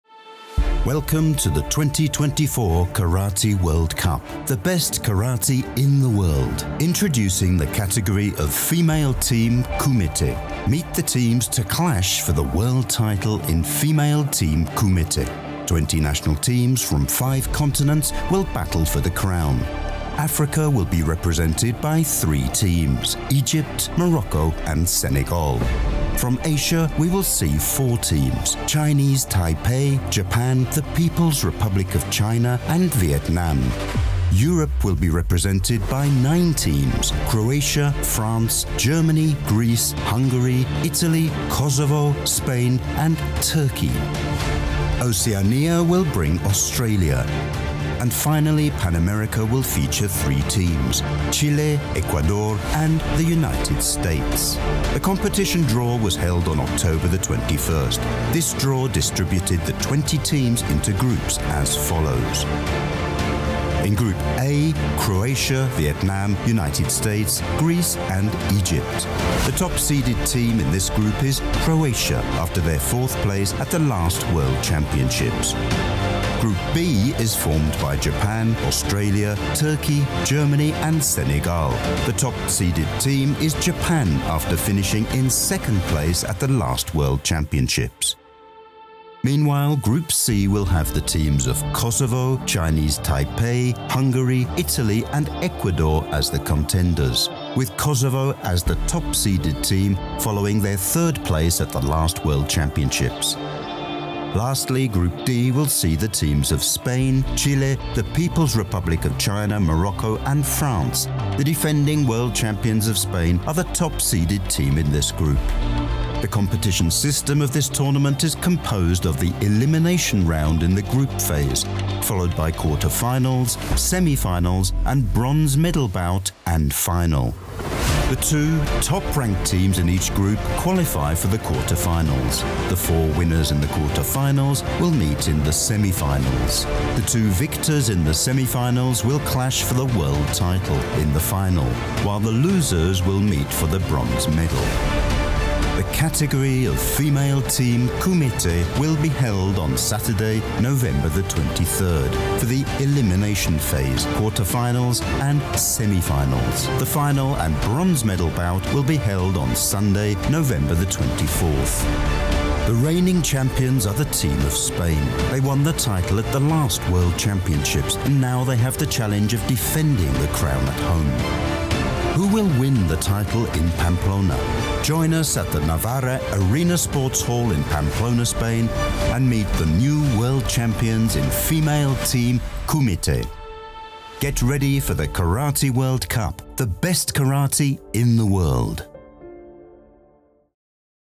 Karate World Cup: Female Team Kumite Group Phase
20 Countries from 5 continents will be in action on the category of Female Team Kumite at the Karate World Cup. Watch ALL of the teams that will be competing to take home the trophy this upcoming November 22-24 at Pamplona, Spain.